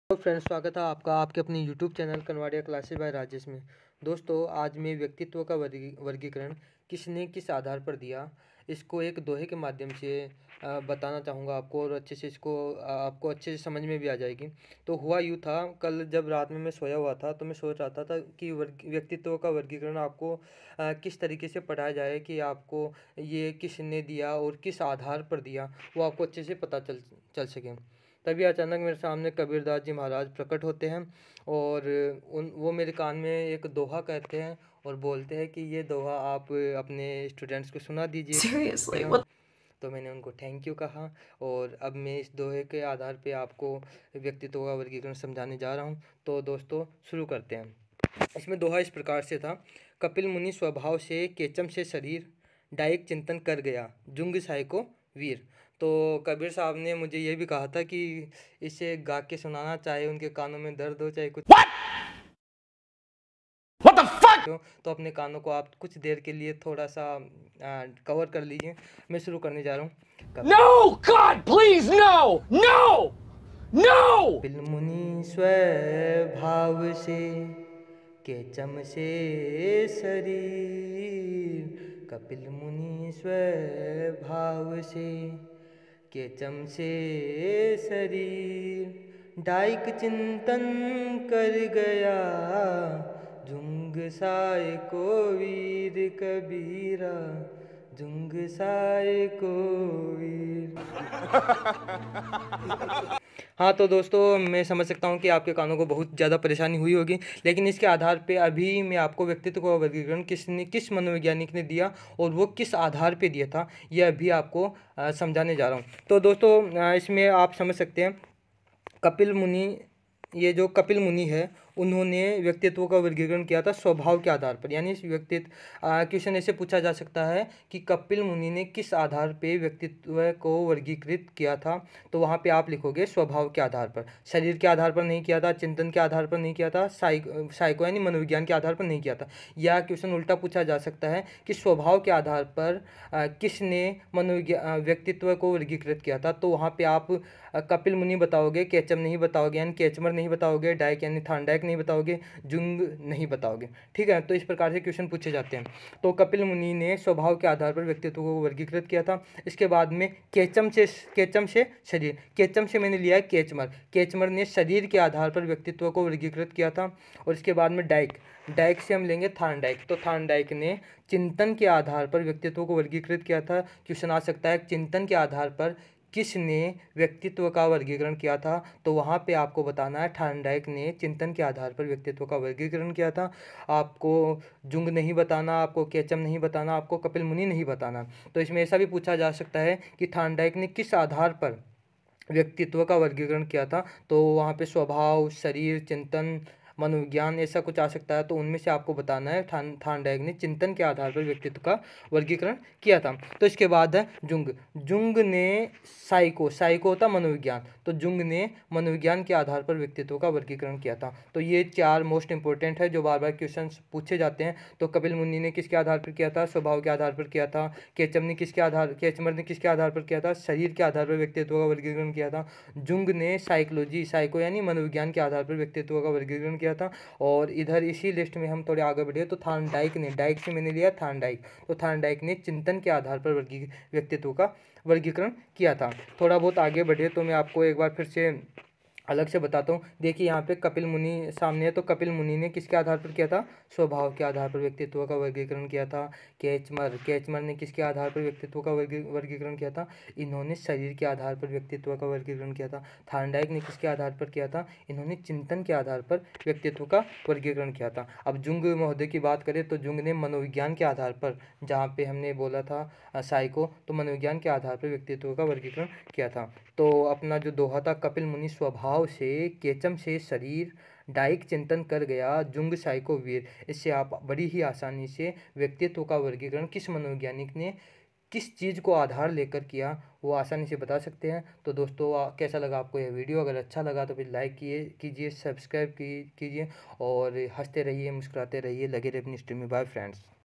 कबीर जी के दोहे से व्यक्तित्व का वर्गीकरण
व्यक्तित्व का वर्गीकरण किसने किस आधार पर किया है इस वीडियो में इन सब चीजों को ट्रिक के साथ याद करने की कोशिश करवाने की कोशिश की है